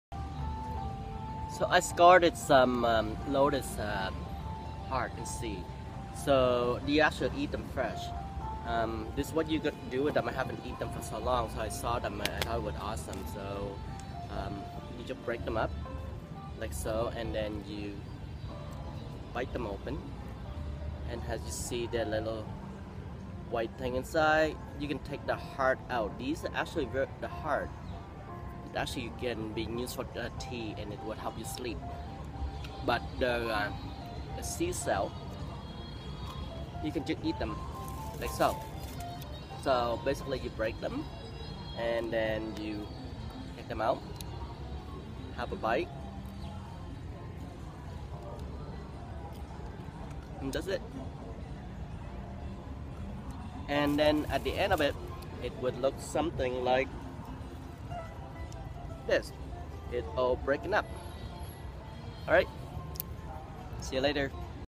How to eat lotus heart - Foodie Adventure in Ayutthaya
Street Foodie Adventure in Ayutthaya Thailand